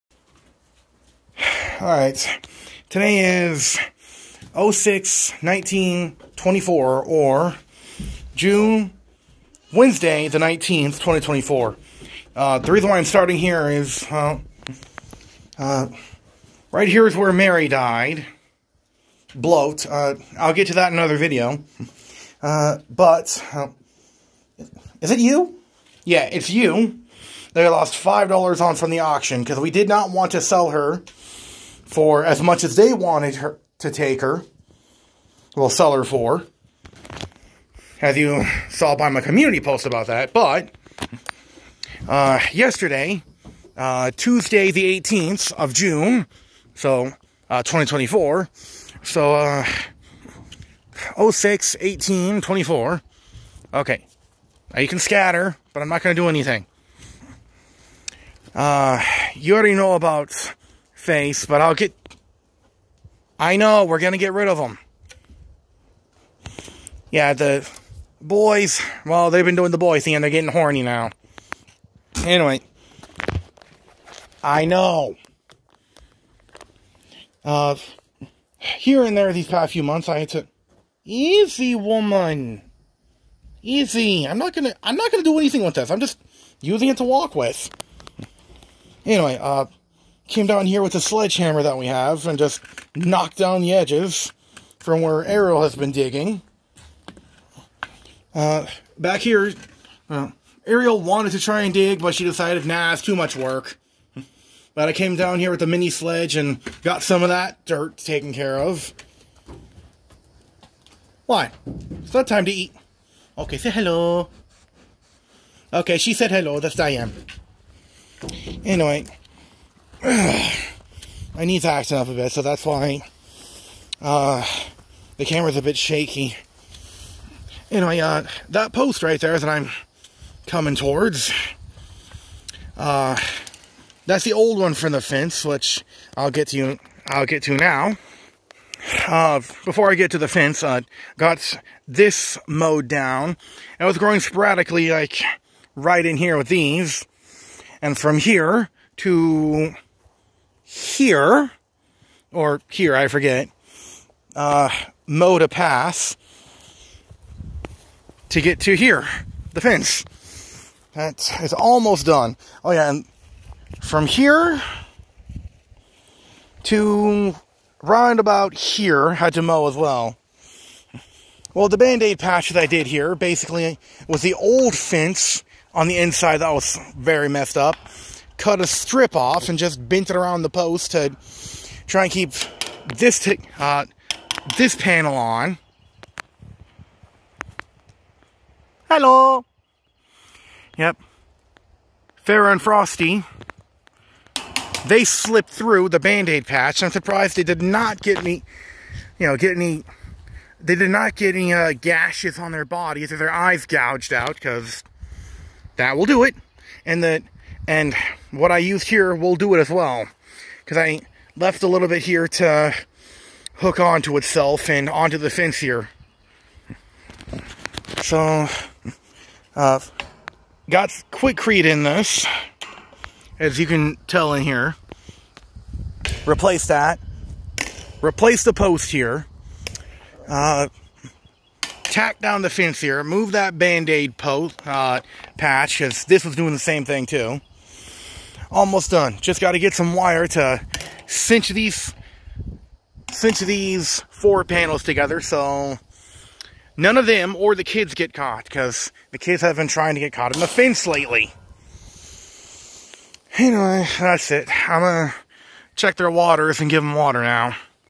farm update 06/19/24 fenced in problems & herd counting down
It took a few months of saving to get the stuff to fix the fence & a 100F day to fix it but the fence is fixed now just gotta get some wire to synch up the gap tho

I'm thankful the boys didn't get hurt or any of the kids get pinned in it either